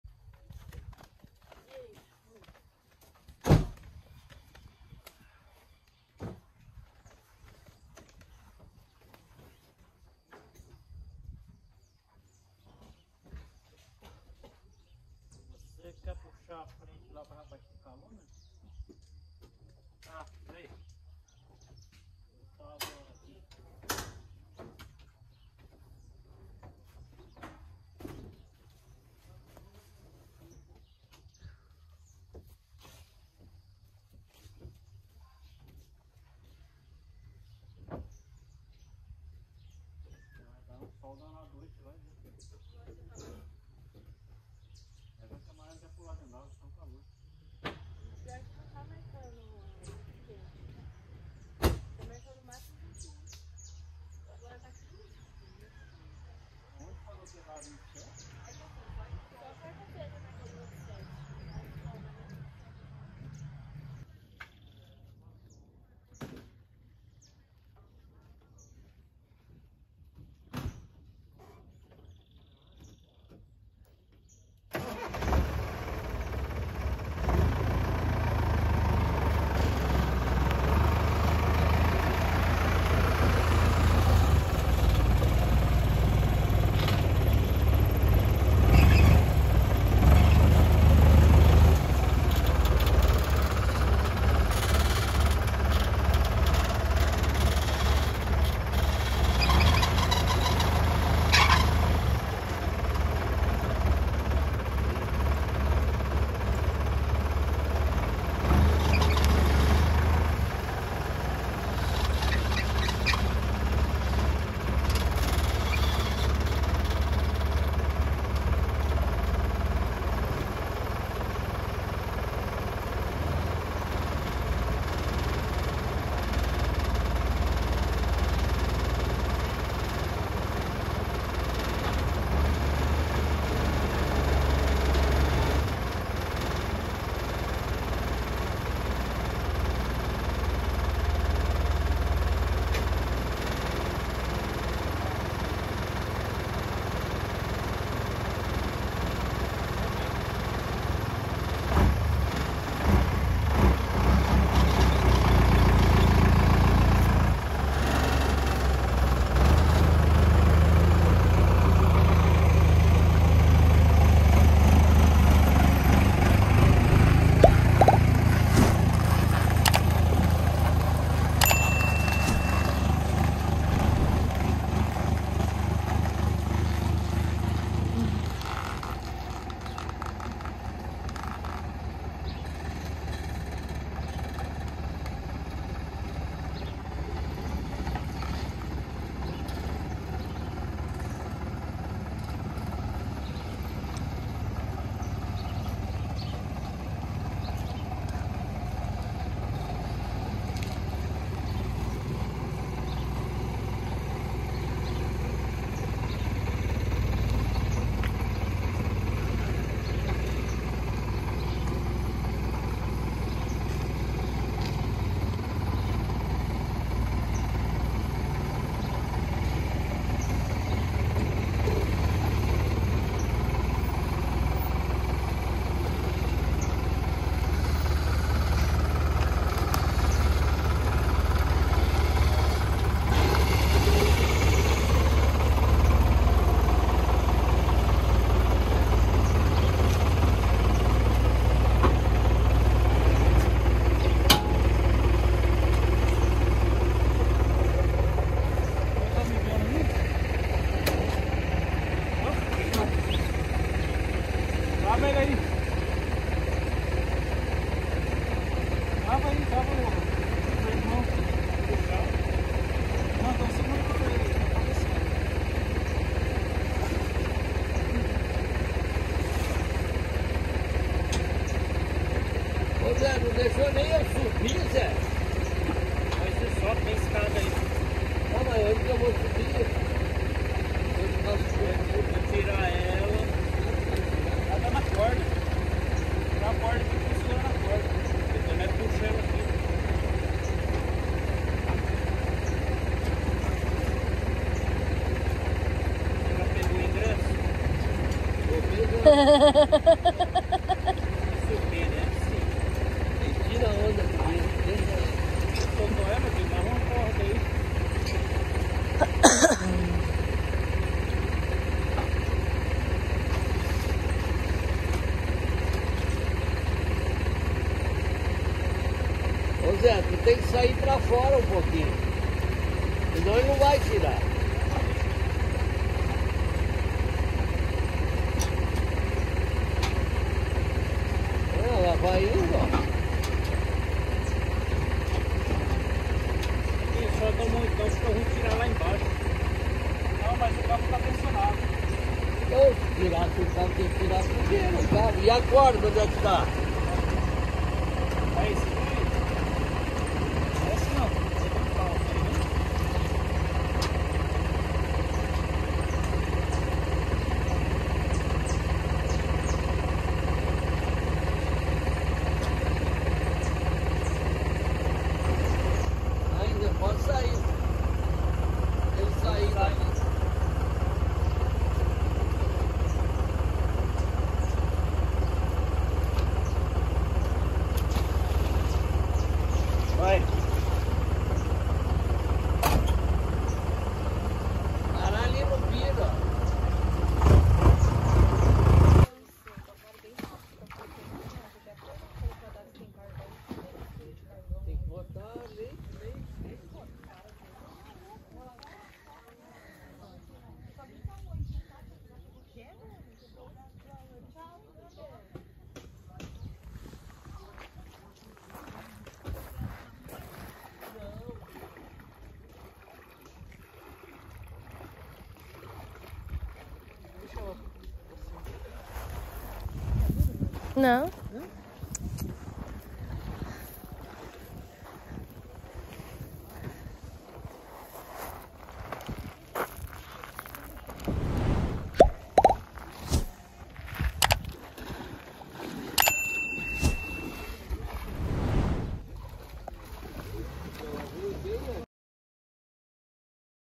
Colocando a danada na água!!!
Mais um domingão de passeio por aqui e colocamos ela na água pra curtir mais um dia em família 🙏🏼🙏🏼🙌🏼